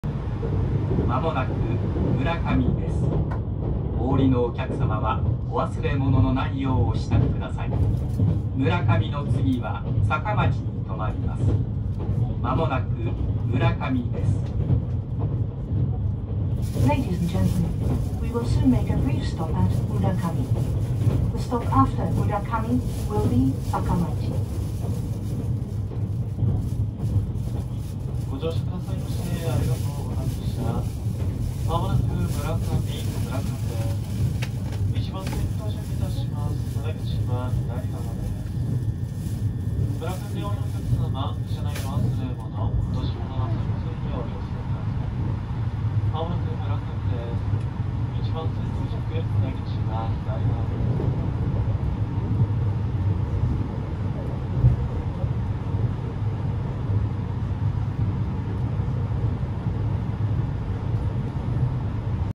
いなほ14号 車内案内と室内灯
余目から村上駅へ向かう途中で室内灯が消えます！